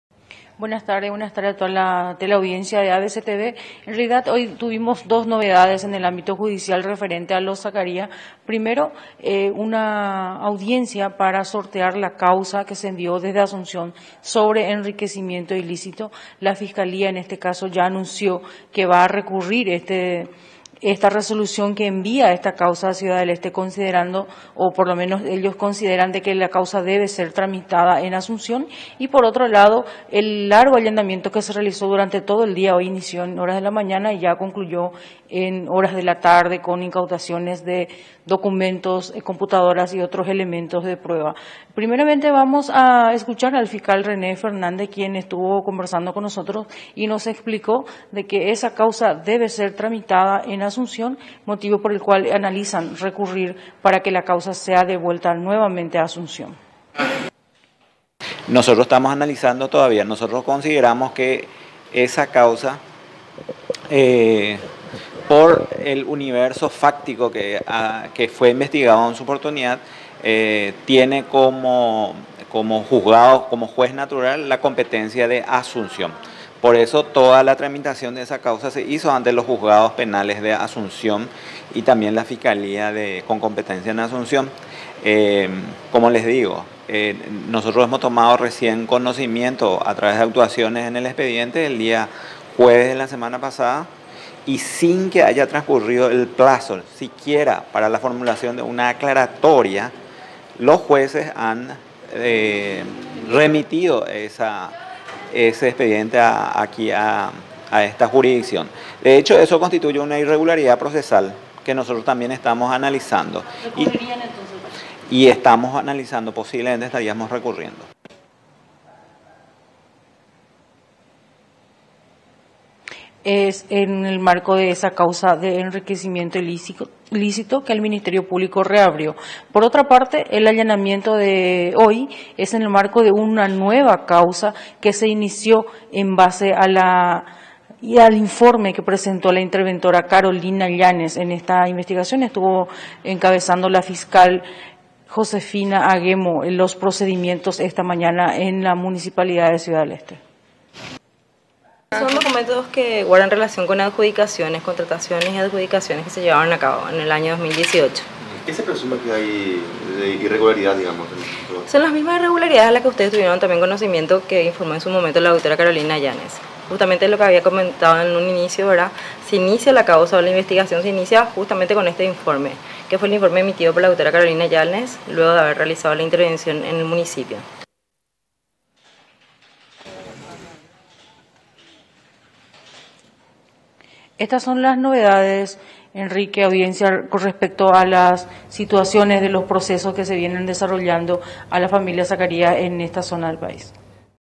CDE: Fiscalía allanó oficinas de la comuna
La fiscala Josefina Aghemo y Osmar Legal, junto con agentes de la Policía de Hechos Punibles Económicos y Financieros, allanan desde este mediodía varias oficinas de la Municipalidad de Ciudad del Este.